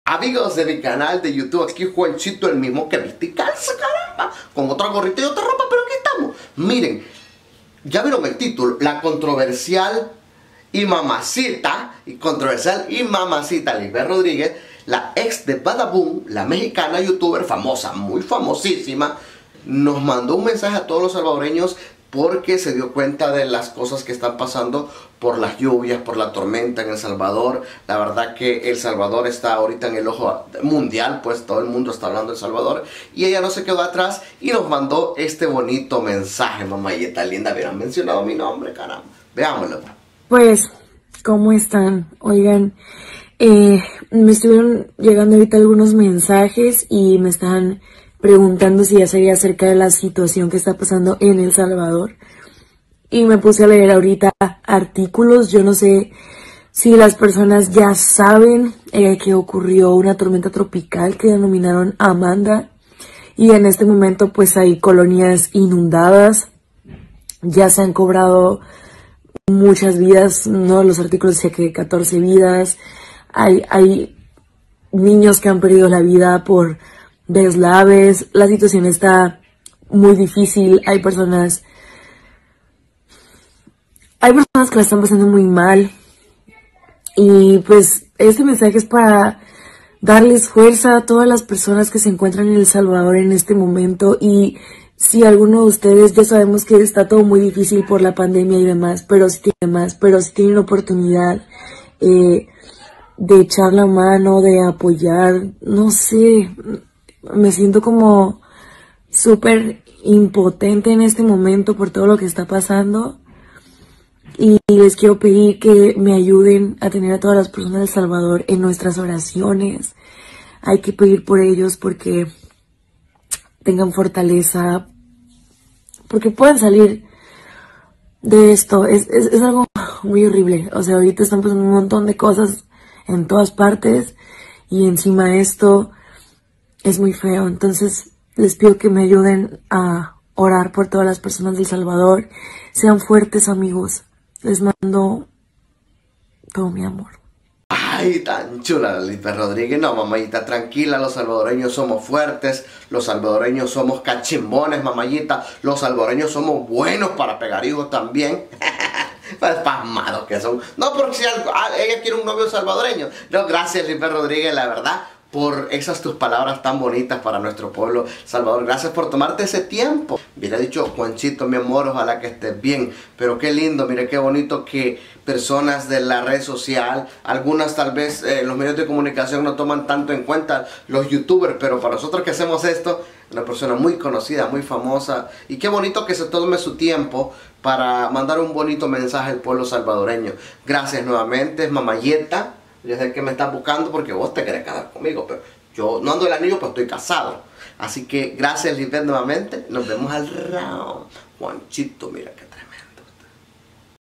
Lisbeth Rodriguez Y su Mensaje Para el Pueblo Salvadoreño
LA MAMASITA DE ATRAPANDO INFIELES LisbethRodriguez NOS MANDA ESTE MENSAJE